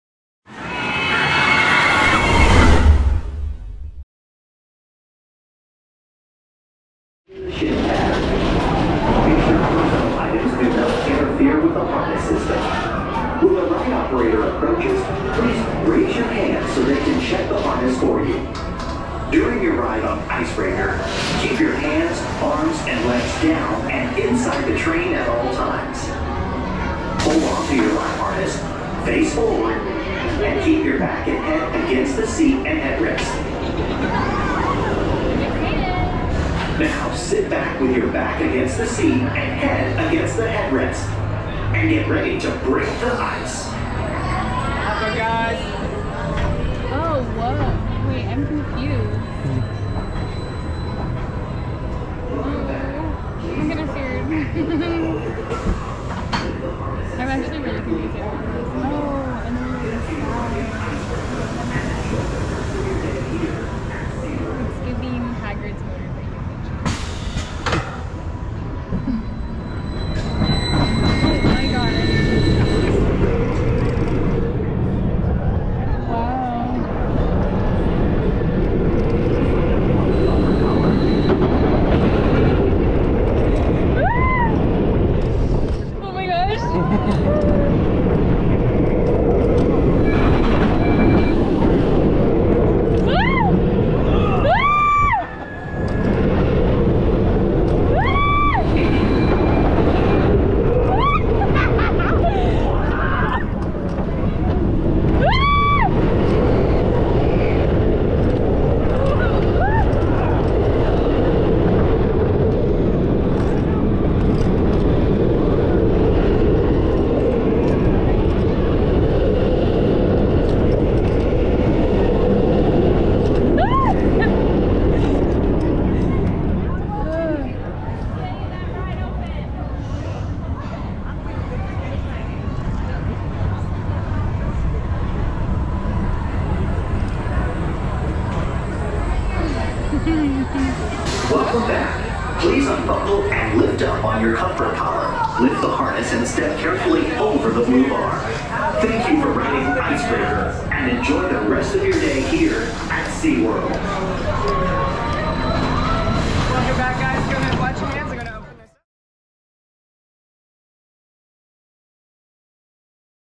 Ice Breaker front seat on-ride 5K POV @ 60fps SeaWorld Orlando
Named after the icy Arctic summits, this new-for-2022 custom designed Premier Rides Sky Rocket coaster at SeaWorld Orlando features four airtime filled launches, both backwards and forwards, culminating in a reverse launch into the steepest beyond vertical drop in the state - a 93 feet (28m) tall spike with 100° angle. The thrills continue as riders fly over a near vertical tophat maneuver into a series of exciting twists, turns, and airtime hills at speeds reaching up to 52 mph (83,5 km/h) along 1,900 feet (579m) of track.
Check out the other on-ride POV video we posted to see what it's like to ride in the back seat of this thrilling coaster
You can also watch the off-ride video we posted to see more of what the coaster looks like and how it works
This SeaWorld park is located in Orlando, Florida.